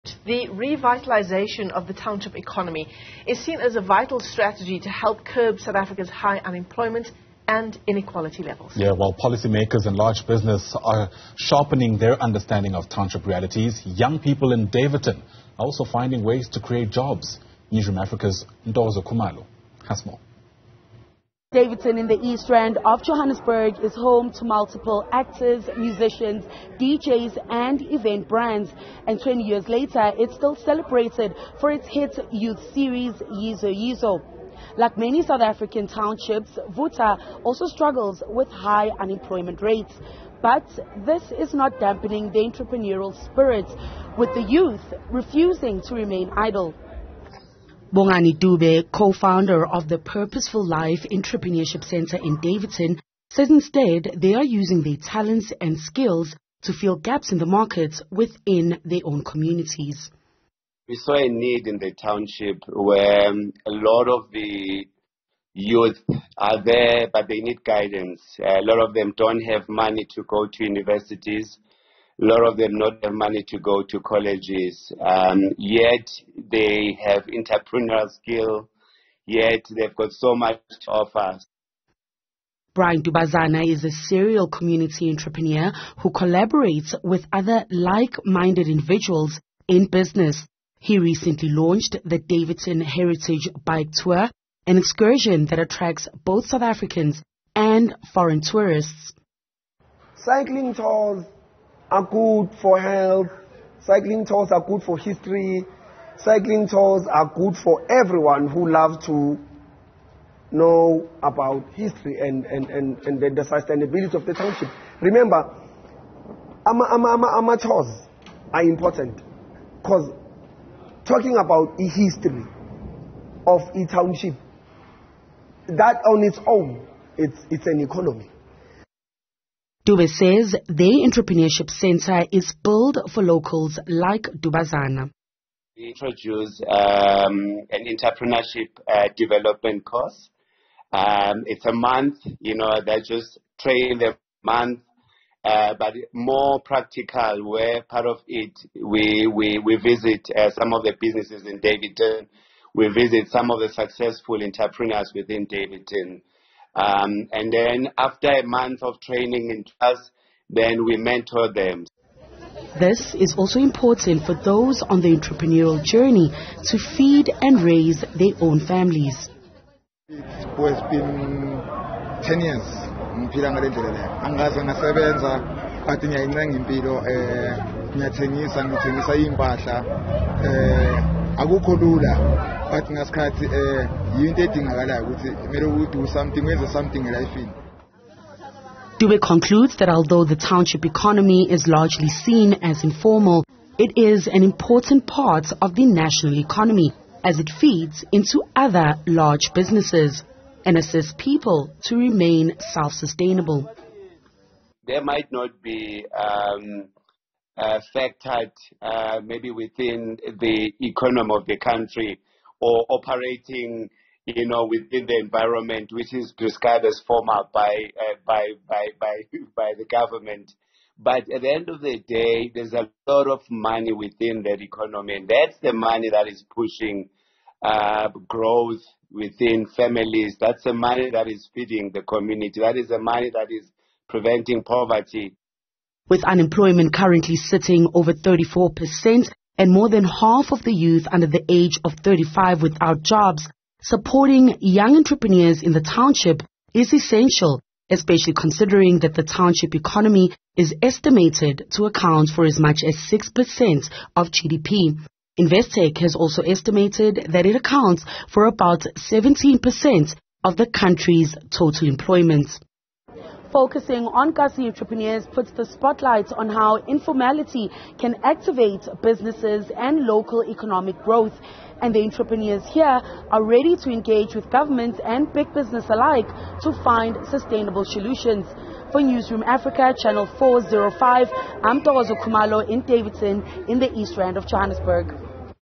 Daveyton youth create own economic opportunities
The revitalisation of the township economy is seen as a vital strategy
to help curb South Africa's high unemployment and inequality levels.
While policymakers and large businesses are sharpening their understanding of township realities, young people in Daveyton are finding ways to create jobs.

Newzroom Afrika’s Ntokozo Khumalo has more.